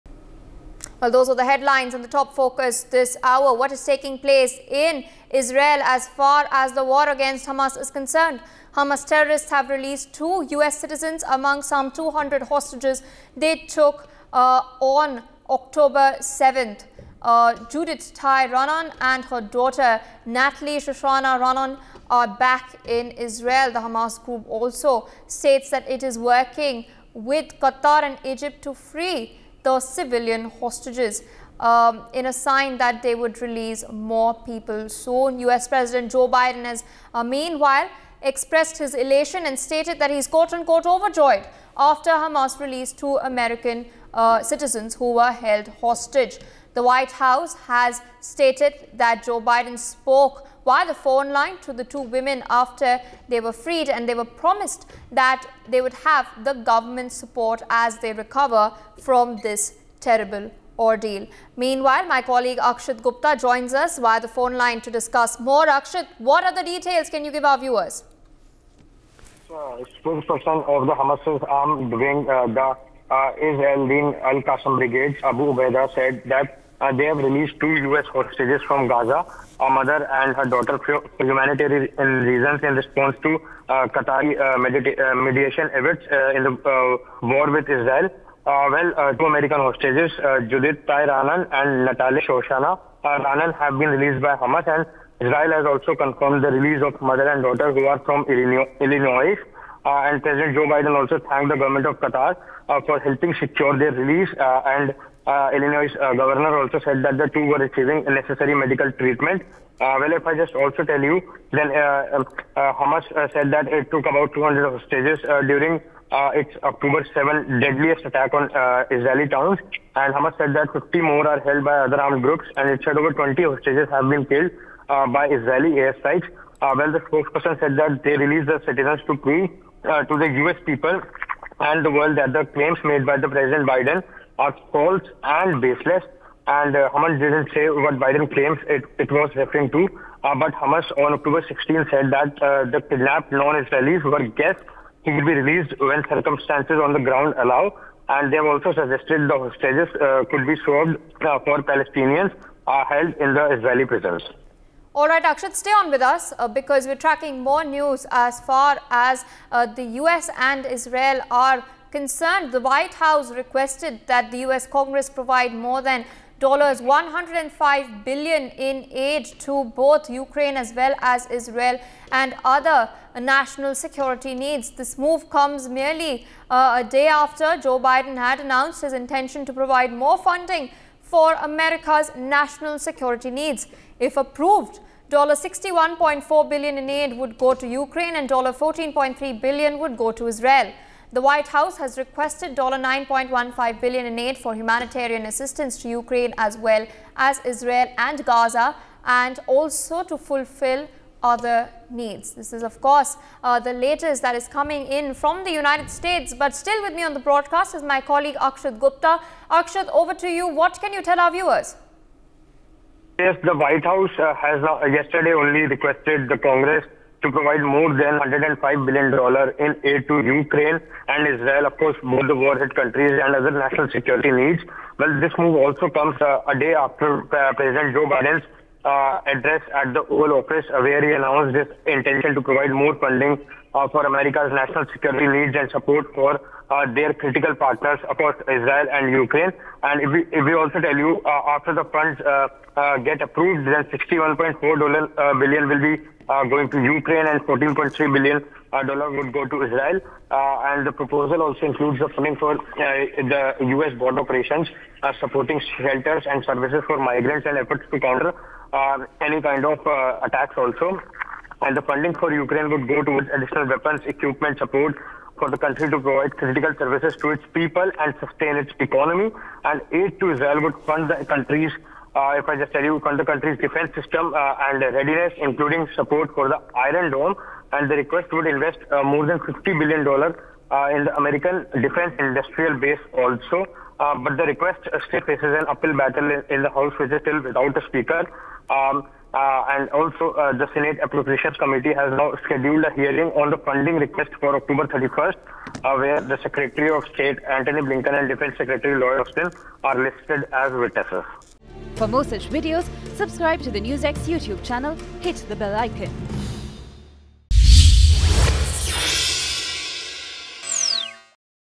2 American Hostages Released | Biden Speaks To Hostages | NewsX
2 American Hostages Released | Biden Speaks To Hostages | NewsX